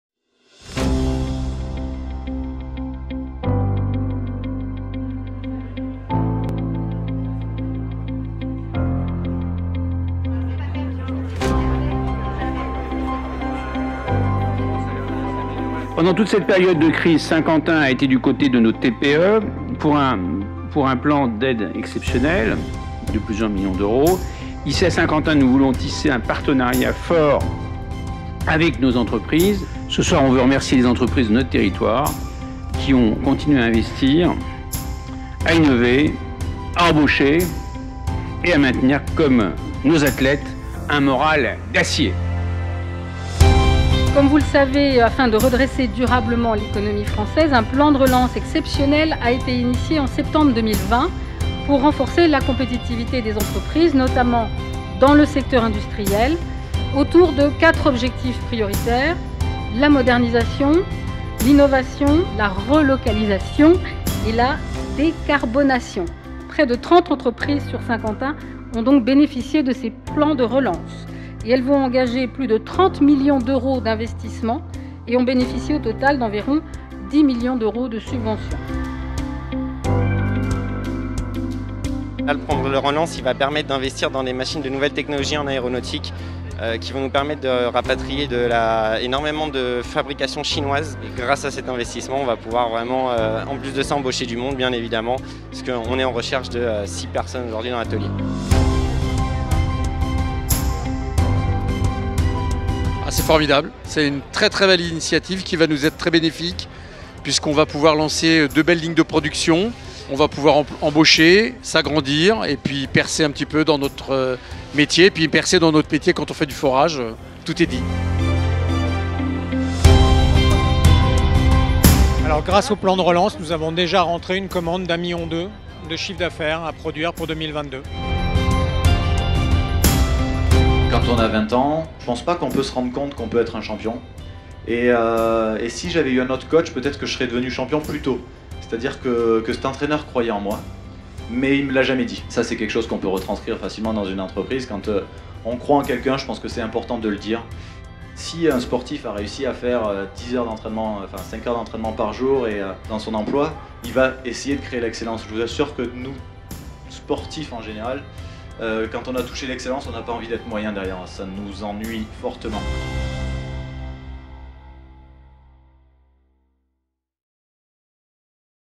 Franc succès pour la 6ème édition du SQY BUSIN€SS DAY
Le SQY BUSIN€SS DAY, la convention d’affaires made in SQY, s’est déroulé jeudi 25 novembre. Les entreprises ont répondu présentes, avec 500 participants et plus de 3200 mises en relation. La veille de cette 6ème édition 100% en ligne, les dirigeants d’entreprises étaient réunis au Vélodrome National pour un warm-up placé sous le signe du Plan de relance et du sport.

Mercredi 24 novembre, avant l’organisation le lendemain des rendez-vous business, Saint-Quentin-en-Yvelines a réuni au Vélodrome National des dirigeants d’entreprises autour des valeurs de performance, de combativité et de ténacité, valeurs communes au milieu économique et au monde du sport. La soirée s’est déroulée autour de 3 temps forts :
- La remise de Trophées aux entreprises qui ont bénéficié des Plans de Relance Etat & Région, et qui ont continué à investir, à innover, et à recruter sur notre territoire
- Un temps d’échange exceptionnel avec Camille Lacourt, champion du monde et d’Europe de natation ; sportif de haut niveau ou dirigeant d’entreprise, l’ambition est la même : la performance et la recherche de l’excellence dans son domaine
- Un temps de networking

Jeudi 25 novembre s’est tenue la 6ème édition du SQY Business Day.

Objectif : favoriser les échanges entre les entreprises du territoire pour booster leur business. En 2020, la crise sanitaire que nous traversions nous avait amené à repenser l’événement et à proposer une convention d’affaires 100% en ligne. Cette année encore, le SQY Business Day se tenait en distanciel ; près de 500 participants se sont connectés et plus de 3200 mises en relation ont pu se faire. Des conférences ont par ailleurs rythmé la journée, et vous pourrez les retrouver dans la playlist "Economie" !